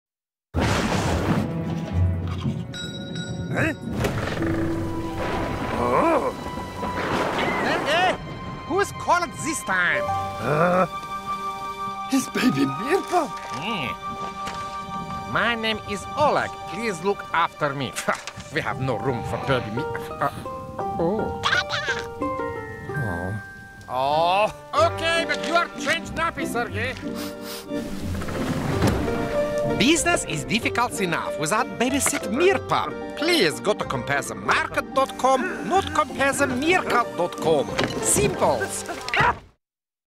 Compare the Meerkat -- Sergei and I find Baby Oleg meerpup on doorstep
On recent dark and stormy night Sergei and I were receive mysterious delivery... And it was not my maggot and mushroom pizza, it was baby meerpup name Oleg.

Baby meerkat has turn mansions life upsides down and has already chew up my new Celine Dion video cassette. I put advert in local Meerkovo paper asking if anyone have misplace meerpup but nobody answer.

Business is difficults enough without babysit meerpup so please, remember, for compare meerkats, come for cheap deal on insurance, please go

Yours,

Aleksandr